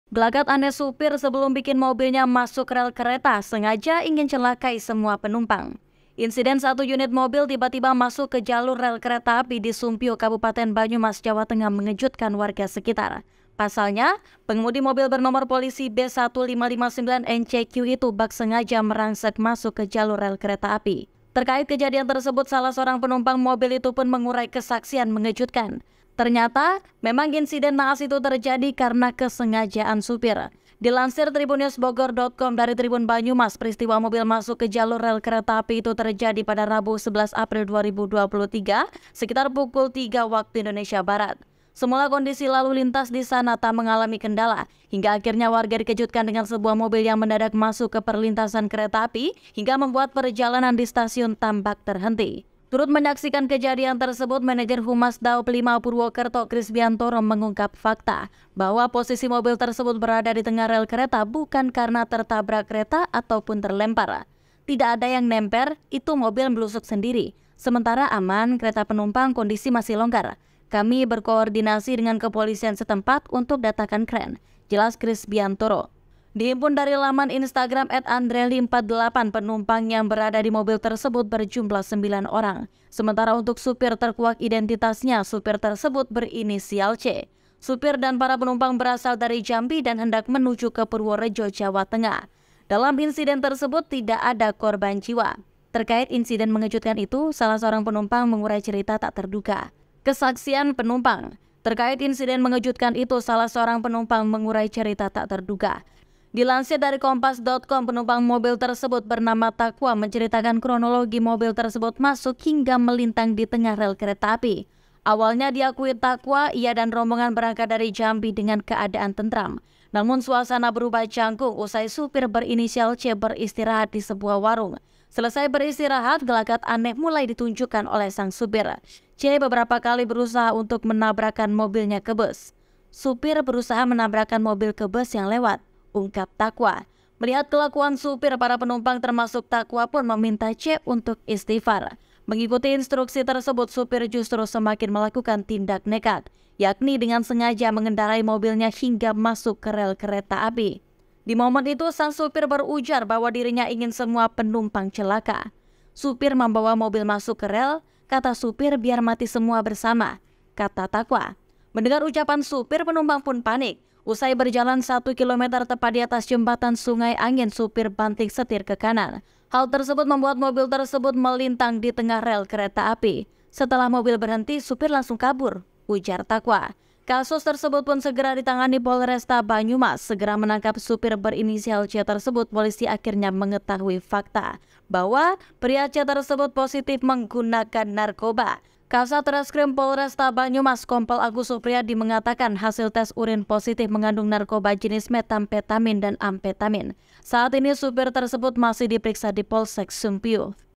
Gelagat Aneh Sopir Sebelum Bikin Mobilnya Masuk Rel Kereta, Sengaja Ingin Celakai Semua Penumpang
TRIBUNBENGKULU.COM - Insiden satu unit mobil tiba-tiba masuk ke jalur rel kereta api di Sumpiuh, Kabupaten Banyumas, Jawa Tengah mengejutkan warga sekitar.